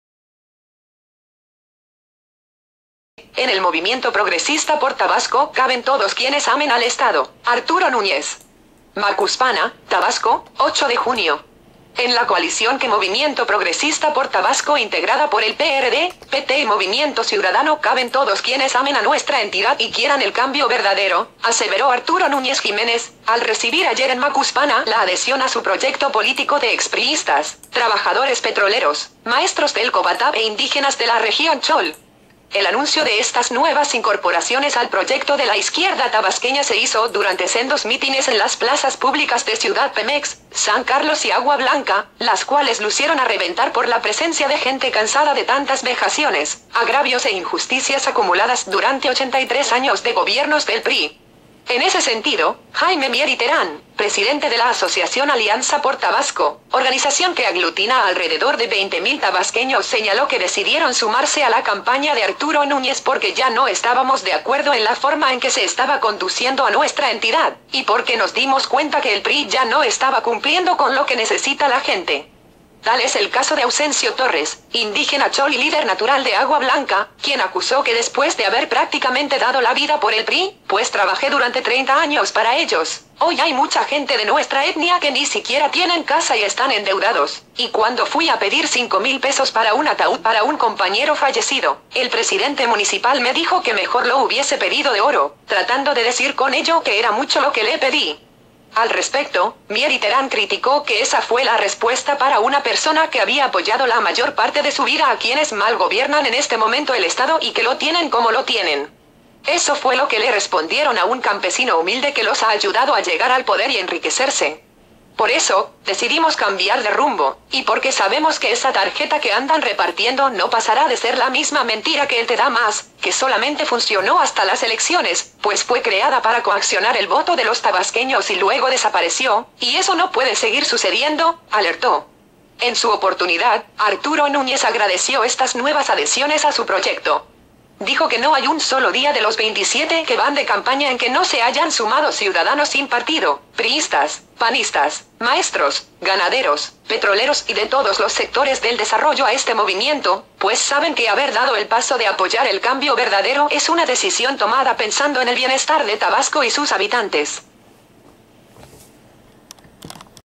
En el Movimiento Progresista por Tabasco caben todos quienes amen al estado, Arturo Núñez.mpg
En el Movimiento Progresista por Tabasco caben todos quienes amen al estado, Arturo Núñez.

Macuspana, Tabasco, 8 de junio.- En la coalición que Movimiento Progresista por Tabasco --integrada por el PRD, PT y Movimiento Ciudadano-- caben todos quienes amen a nuestra entidad y quieran el cambio verdadero, aseveró Arturo Núñez Jiménez, al recibir ayer en Macuspana la adhesión a su proyecto político de ex priístas, trabajadores petroleros, maestros del Cobatab e indígenas de la región chol.

El anuncio de estas nuevas incorporaciones al proyecto de la izquierda tabasqueña se hizo durante sendos mítines en las plazas públicas de Ciudad Pemex, San Carlos y Agua Blanca, las cuales lucieron a reventar por la presencia de gente cansada de tantas vejaciones, agravios e injusticias acumuladas durante 83 años de gobiernos del PRI.

En ese sentido, Jaime Mier y Terán, presidente de la asociación Alianza por Tabasco, --organización que aglutina a alrededor de 20 mil tabasqueños-- señaló que decidieron sumarse a la campaña de Arturo Núñez "porque ya no estábamos de acuerdo en la forma en que se estaba conduciendo a nuestra entidad, y porque nos dimos cuenta que el PRI ya no estaba cumpliendo con lo que necesita la gente".

Tal es el caso de Ausencio Torres, indígena chol y líder natural de Agua Blanca, quien acusó que después de haber "prácticamente dado la vida" por el PRI, pues "trabajé durante 30 años para ellos, hoy hay mucha gente de nuestra etnia que ni siquiera tienen casa y están endeudados, y cuando fui a pedir cinco mil pesos para un ataúd para un compañero fallecido, el presidente municipal me dijo que mejor lo hubiese pedido de oro, tratando de decir con ello que era mucho lo que le pedí".

Al respecto, Mier y Terán criticó que esa fue la respuesta para una persona que había apoyado la mayor parte de su vida "a quienes mal gobiernan en este momento el estado y que lo tienen como lo tienen; eso fue lo que le respondieron a un campesino humilde que los ha ayudado a llegar al poder y enriquecerse".

Por eso, decidimos cambiar de rumbo, y porque sabemos que esa tarjeta que andan repartiendo no pasará de ser la misma mentira que el Te da más, que solamente funcionó hasta las elecciones, pues fue creada para coaccionar el voto de los tabasqueños y luego desapareció, y eso no puede seguir sucediendo, alertó.

En su oportunidad, Arturo Núñez agradeció estas nuevas adhesiones a su proyecto; dijo que no hay un sólo día de los 27 que van de campaña en que no se hayan sumado ciudadanos sin partido, priistas, panistas, maestros, ganaderos, petroleros y de todos los sectores del desarrollo a este movimiento, pues saben que haber dado el paso de apoyar el cambio verdadero es una decisión tomada pensando en el bienestar de Tabasco y sus habitantes.